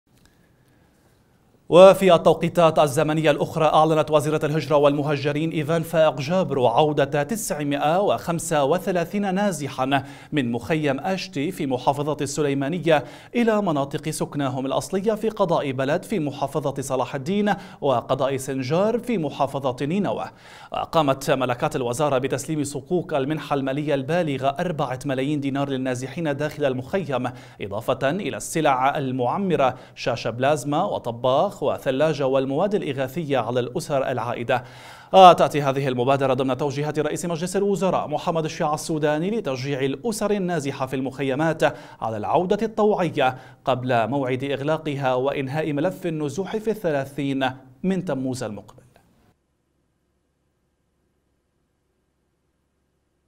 عودة طوعية للنازحين من مخيم آشتي الى بلد
وفي التوقيتات الزمنية الأخرى أعلنت وزيرة الهجرة والمهجرين إيڨان فائق جابرو عودة تسعمئه وخمسة وثلاثين نازحاً من مخيم اشتي في محافظة السليمانية إلى مناطق سكناهم الأصلية في قضاء بلد بمحافظة صلاح الدين وقضاء سنجار بمحافظة نينوى، و قامت ملاكات الوزارة بتسليم صكوك المنحة المالية البالغة أربعة ملايين دينار للنازحين داخل المخيم، إضافة إلى السلع المعمرة شاشة بلازما وطباخ وثلاجة والمواد الإغاثية على الأسر العائدة، وتأتي هذه المبادرة ضمن توجيهات رئيس مجلس الوزراء محمد شياع السوداني لتشجيع الأسر النازحة في المخيمات على العودة الطوعية قبل موعد إغلاقها وانهاء ملف النزوح في الثلاثين من تموز المقبل.